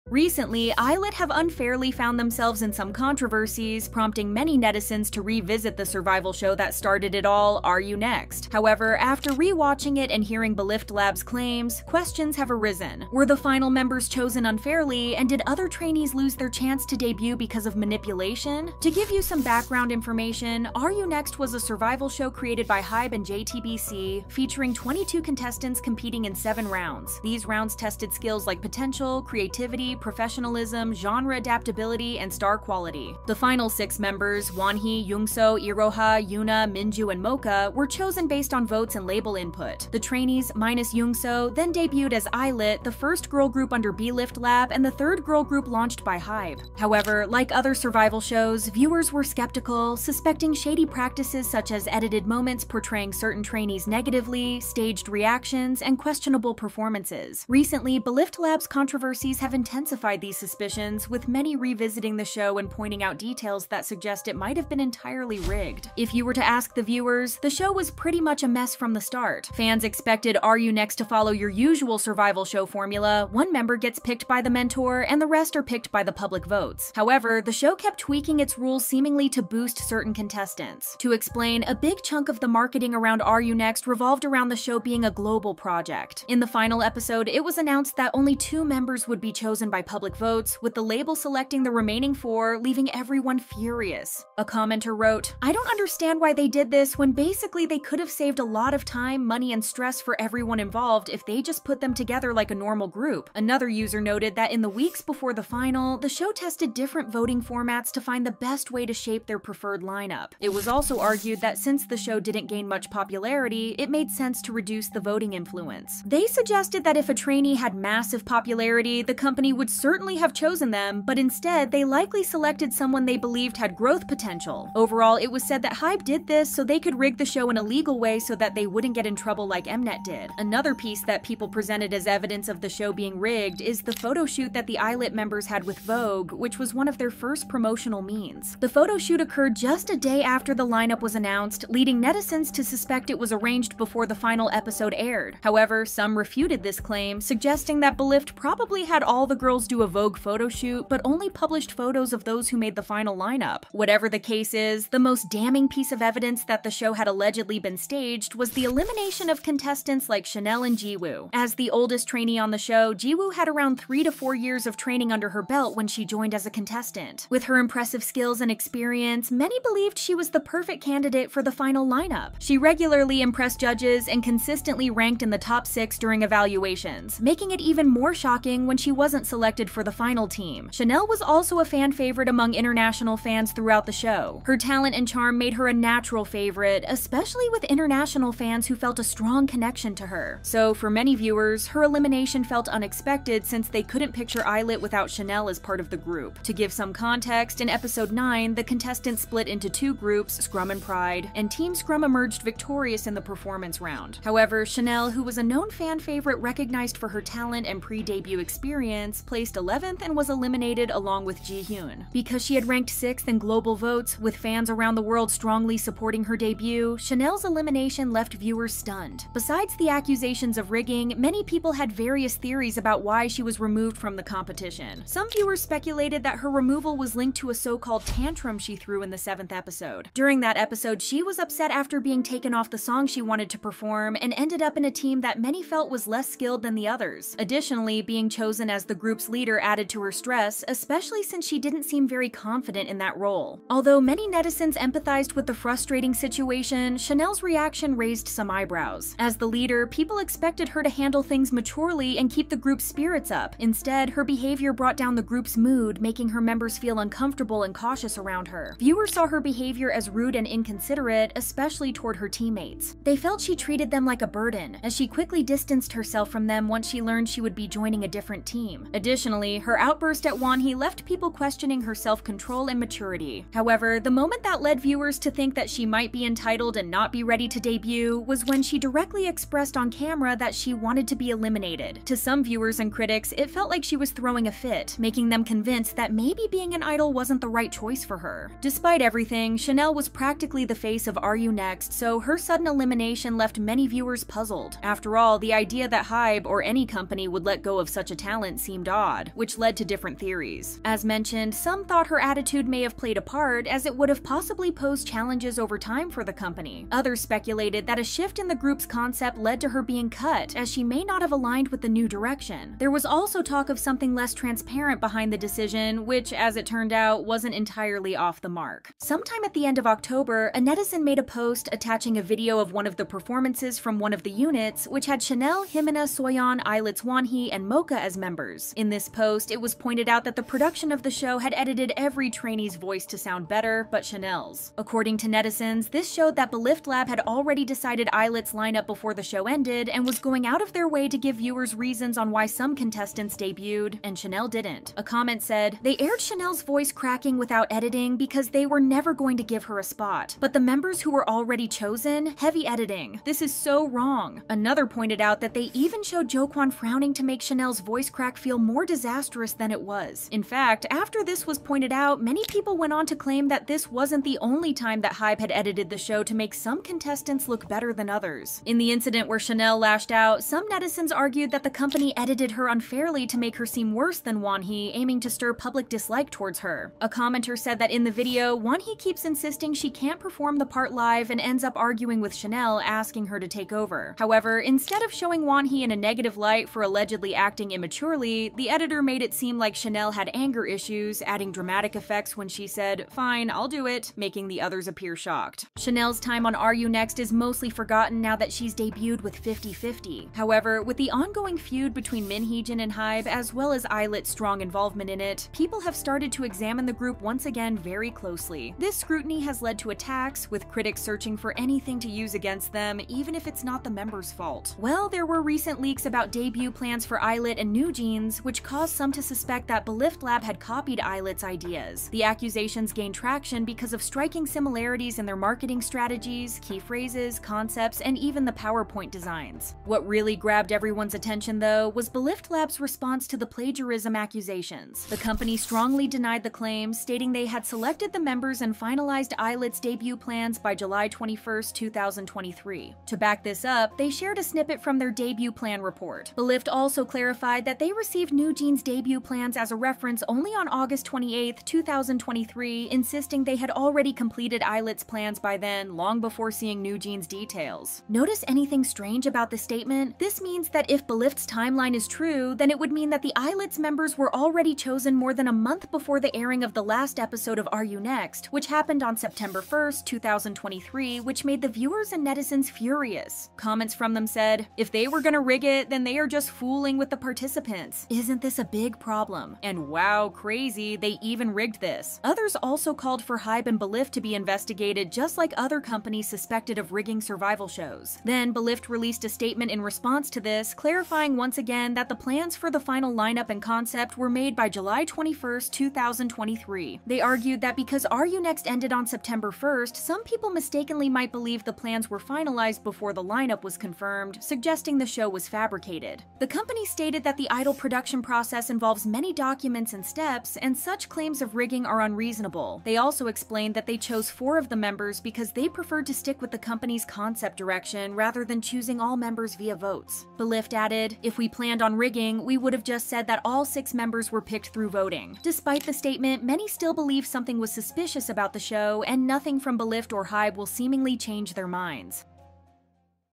The Untold Truth About ILLIT's Lineup
ILLIT recently faced backlash, leading netizens to revisit R U Next?. Now, many are wondering if the final lineup was unfairly decided and if other trainees missed their debut due to manipulation.